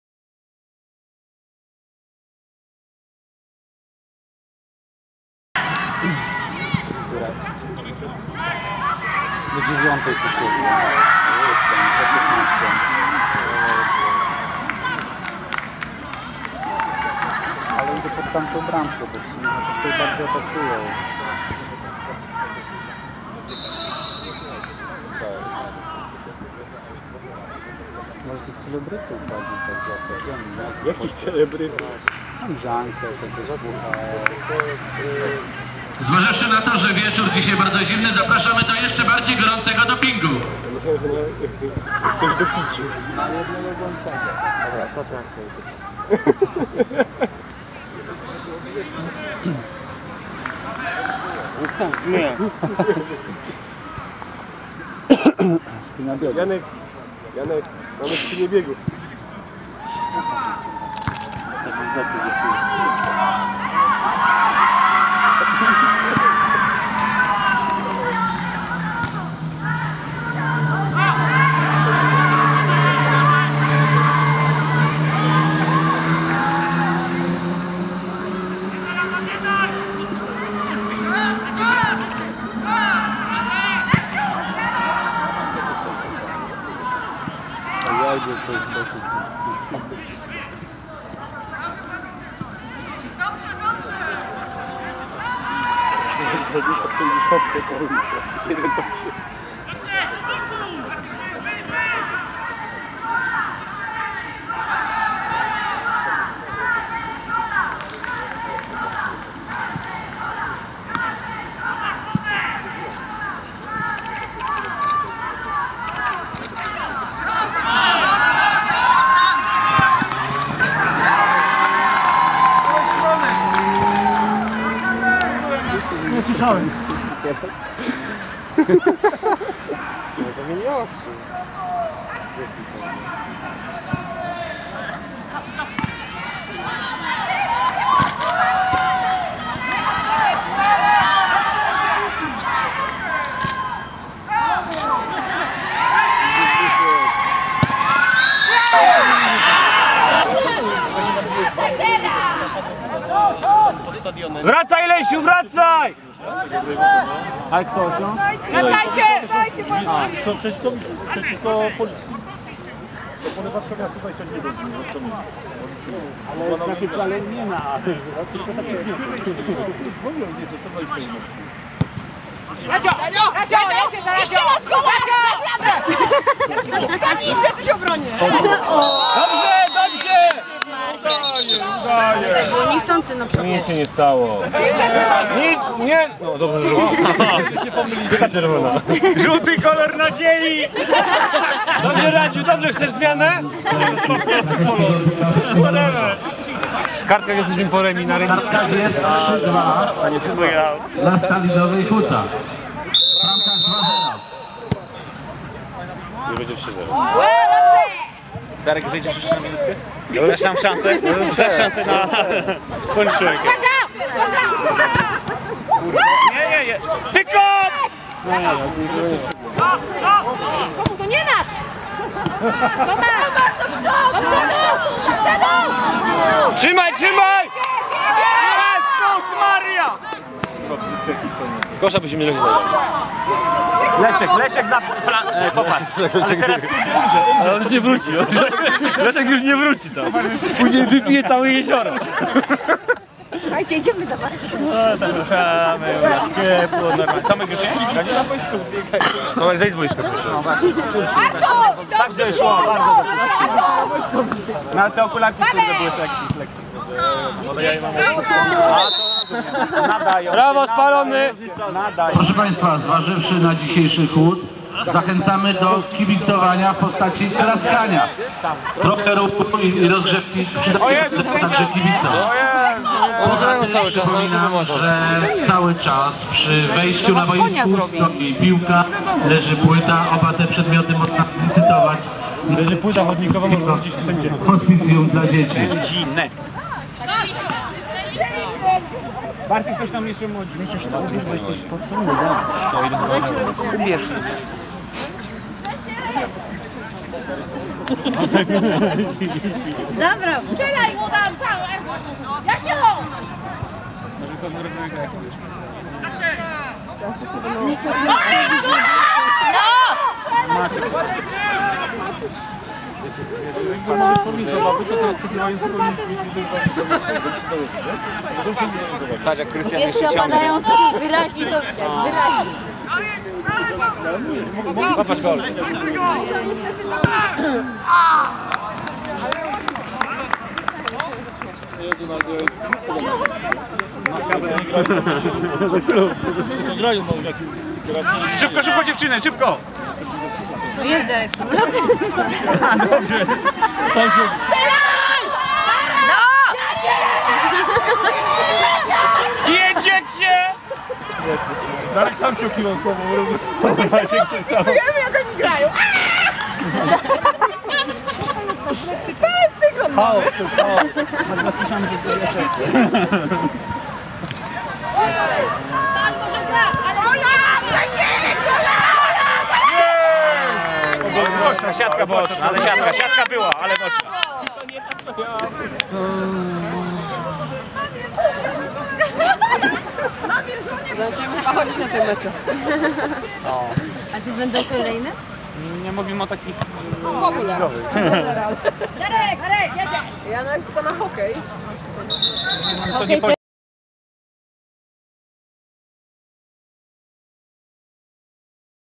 Mecz Charytatywny Krakowskie Biuro Festiwalowe vs. Stal Nowa Huta
Mecz Charytatywny na rzecz Fundacji Małopolskiego Hospicjum dla Dzieci.